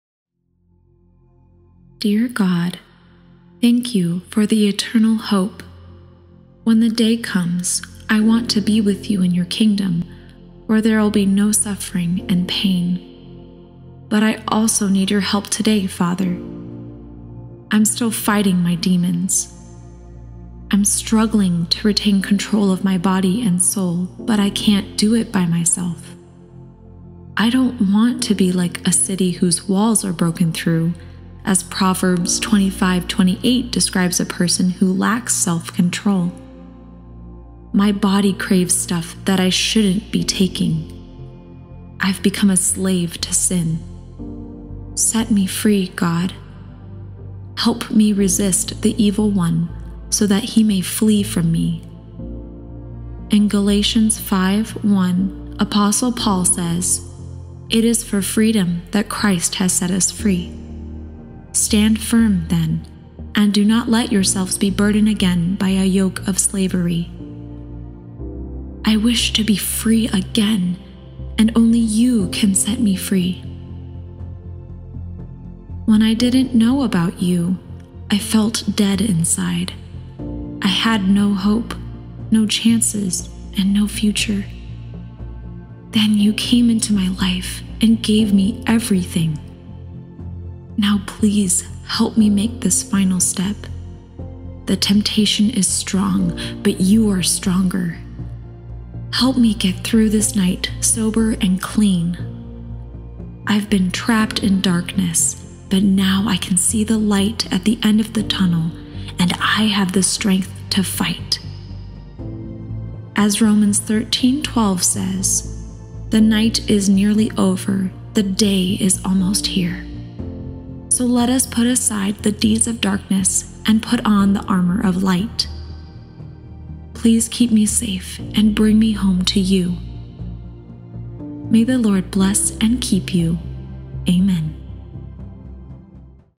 Prayer for Opioids, Drugs and Substance Addicts | Addiction Prayer
Prayer for opioids, drugs, and substance addicts. Pray that God frees you from sins and helps you walk in the light and never fall into temptation again. Nothing is impossible to God if you seek his help.

Prayer:
Dear God,
Thank you for the eternal hope. When the day comes, I want to be with you in your kingdom, where there’ll be no suffering and pain. But I also need your help today, Father. I’m still fighting my demons. I’m struggling to retain control of my body and soul but I can’t do it by myself. I don’t want to be “like a city whose walls are broken through,” as Proverbs 25:28 describes a person who lacks self-control. My body craves stuff that I shouldn’t be taking. I’ve become a slave to sin. Set me free, God. Help me resist the evil one, so that he may flee from me. In Galatians 5:1, Apostle Paul says: It is for freedom that Christ has set us free. Stand firm, then, and do not let yourselves be burdened again by a yoke of slavery. I wish to be free again, and only you can set me free.
When I didn’t know about you, I felt dead inside. I had no hope, no chances, and no future. Then you came into my life and gave me everything. Now, please help me make this final step. The temptation is strong, but you’re stronger. Help me get through this night sober and clean. I’ve been trapped in darkness, but now I can see the light at the end of the tunnel, and I have the strength to fight. As Romans 13:12 says: The night is nearly over; the day is almost here. So let us put aside the deeds of darkness and put on the armor of light. Please keep me safe and bring me home to you.
May the Lord bless and keep you.
Amen!